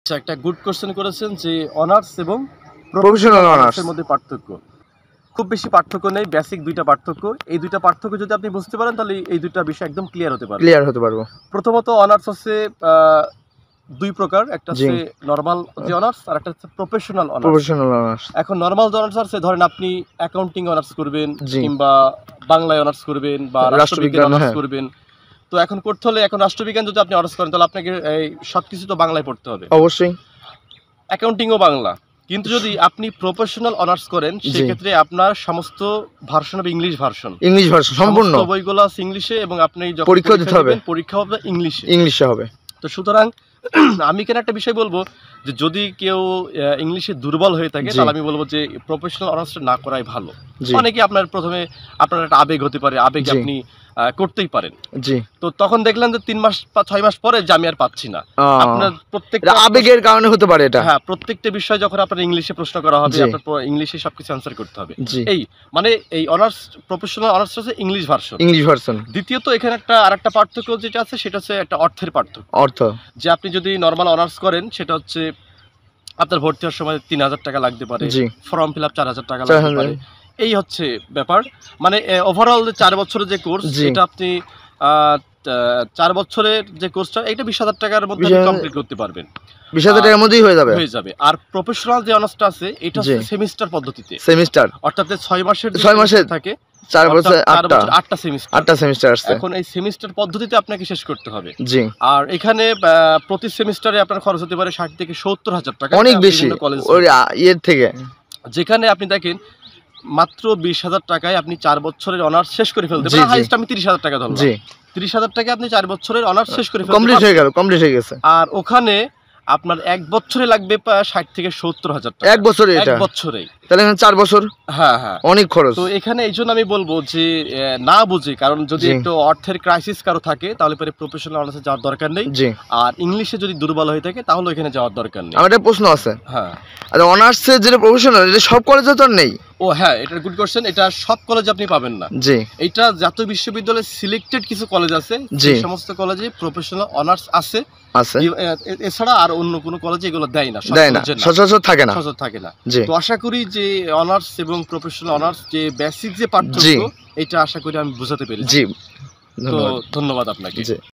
অনার্স বনাম প্রফেশনাল অনার্স | Honours VS Professional Honours | National University
In this video I have provided a lot of important information about Honours VS Professional Honours, If you watch this video from start to finish I hope you will benefit a lot, stay with us all the time, thank you

Title - Honours VS Professional Honours
Uploaded on - Youtube
Copyright - Bangla Express